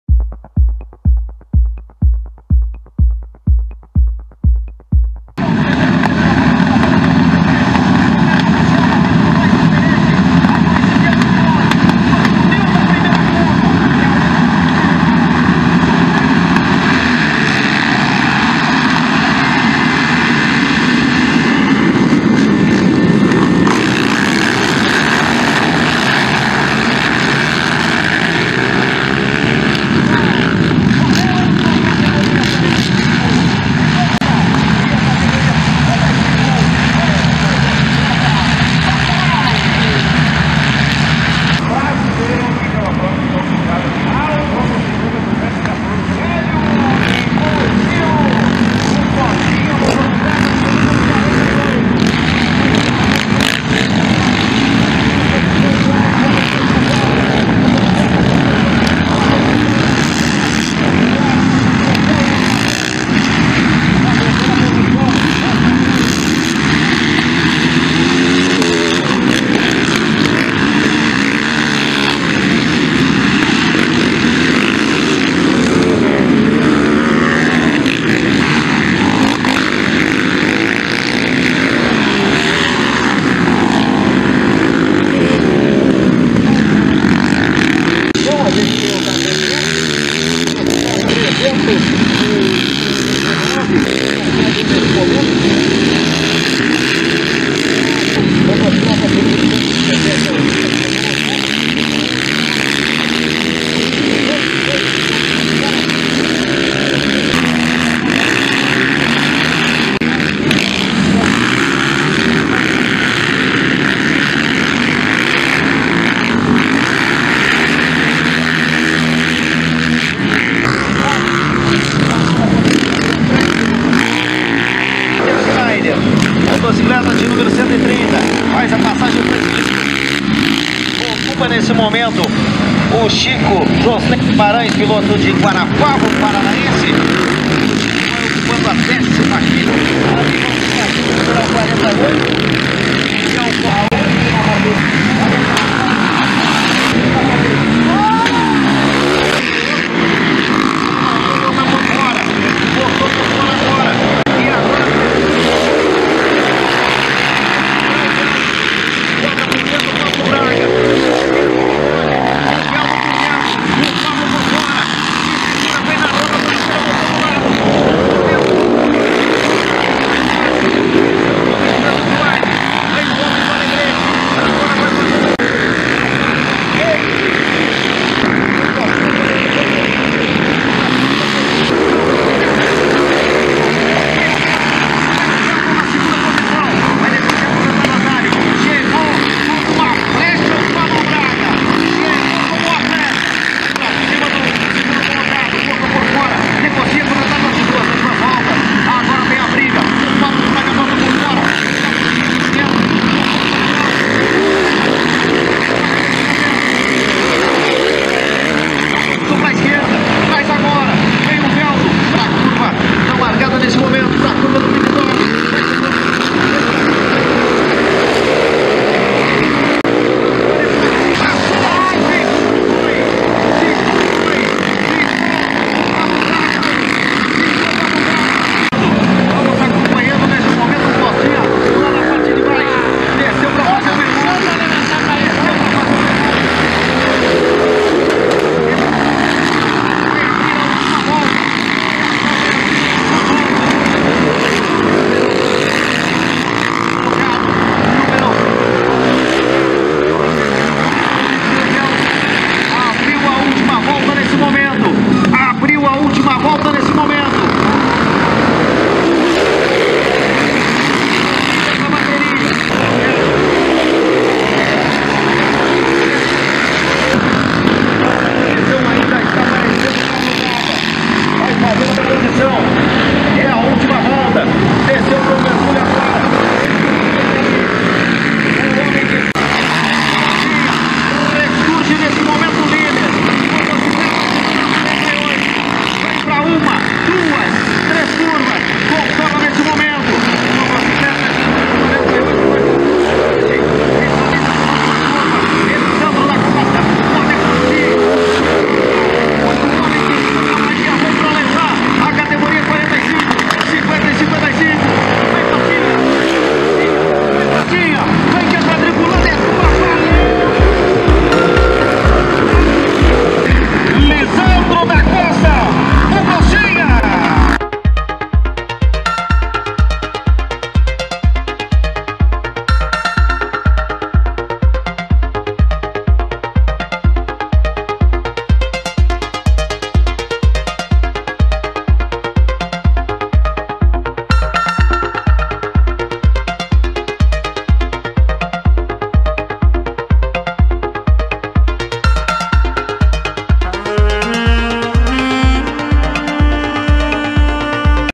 VX Nacional 45, 50 e 55 pelo Brasileiro de Velocross 2024 no CT Guga 2024
Bateria disputada em Salto del Guairá no CT Guga 214